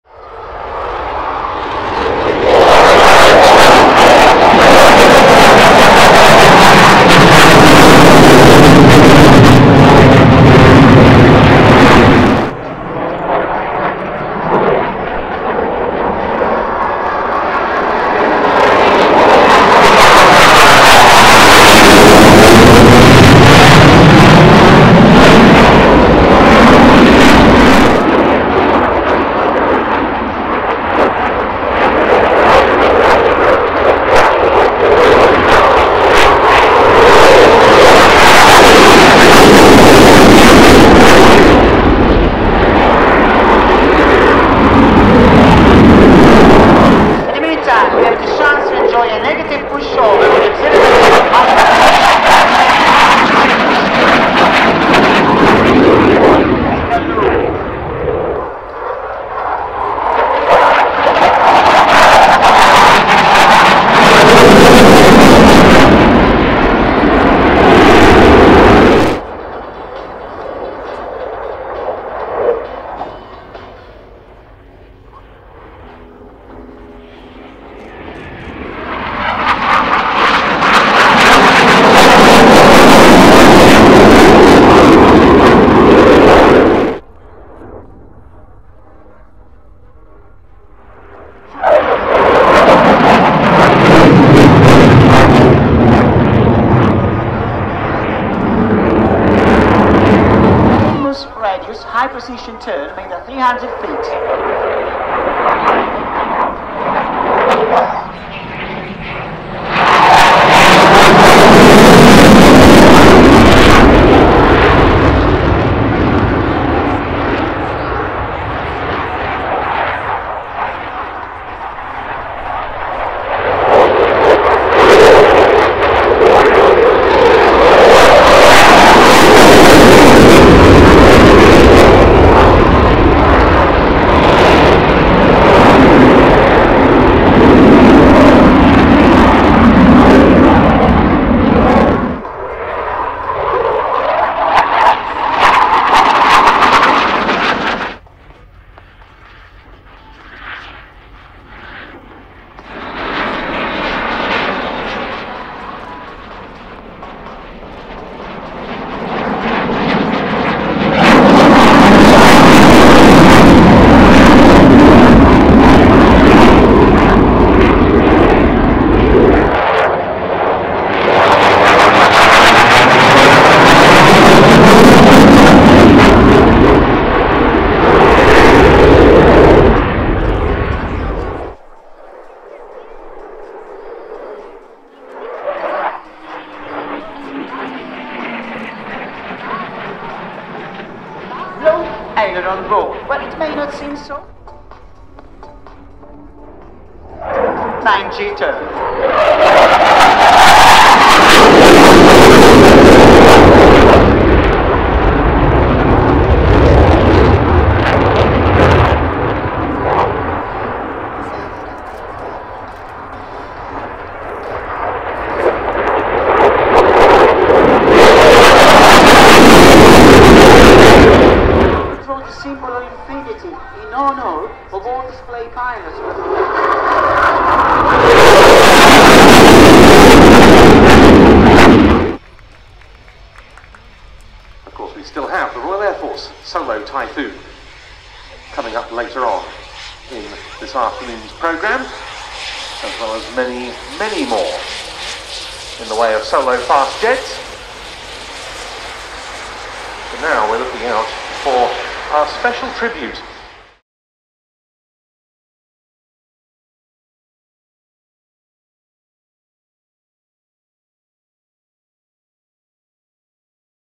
Italian Eurofighter Typhoon Solo Display - RIAT 2024
The Italian Air Force Eurofighter Typhoon displaying at the Royal International Air Tattoo 2024. The Italians always put on a fantastic display. This footage was shot handheld using a Panasonic HC-X20E

Chapters:

00:00 Display
03:52 Taxi
04:13 End screen